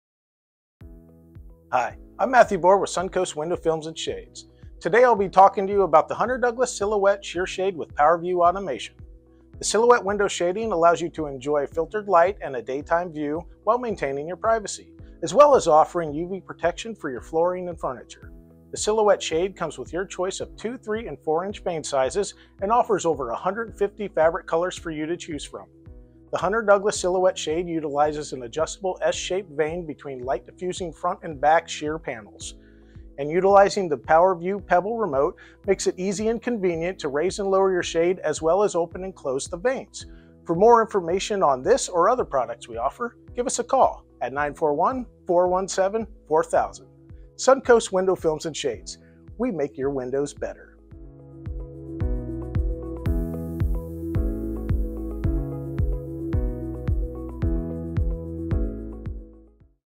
8 Benefits of Hunter Douglas Silhouette Sheer Shades
Learn how Hunter Douglas Silhouette Window Shadings let you enjoy filtered light and a daytime view, while maintaining your privacy and energy savings with just the push of a button.